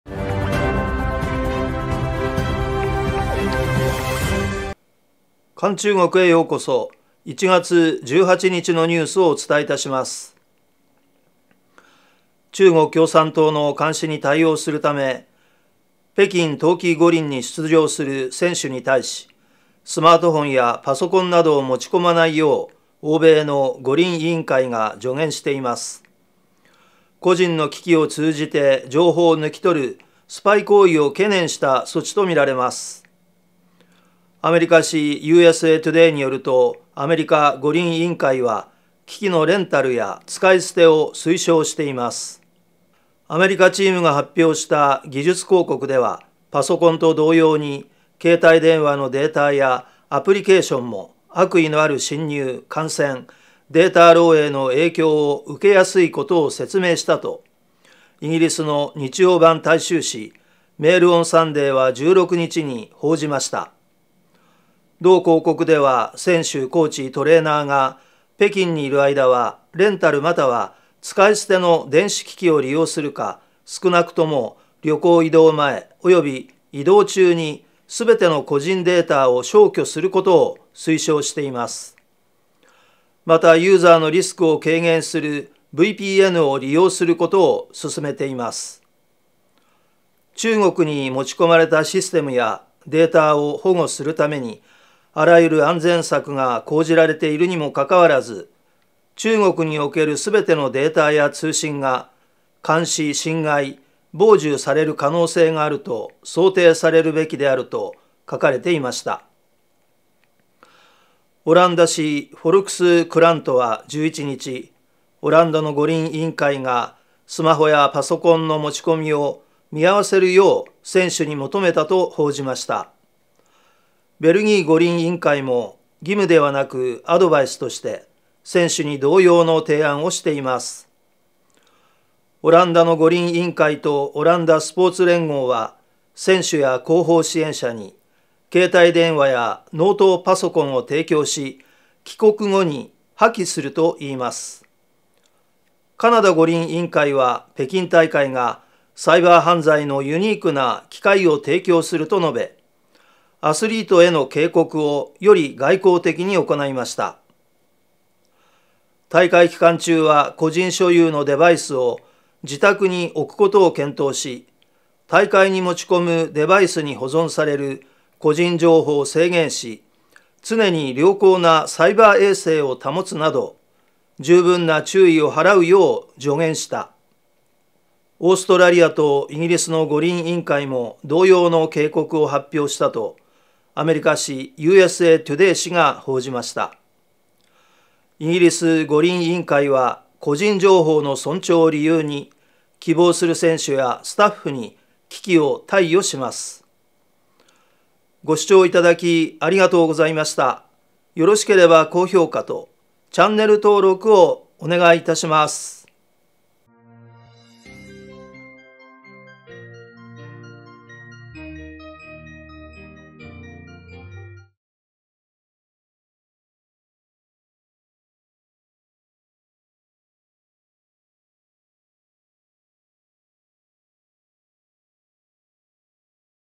冬季五輪での中共の監視に懸念 欧米の対応は？
国共産党（以下、中共）の監視に応えるため、北京冬季五輪に出場する選手に対し、スマートフォンやパソコンなどを持ち込まないよう欧米の五輪委員会が助言している。個人の機器を通じて情報を抜き取るスパイ行為を懸念した措置とみられる。

◆関連SNS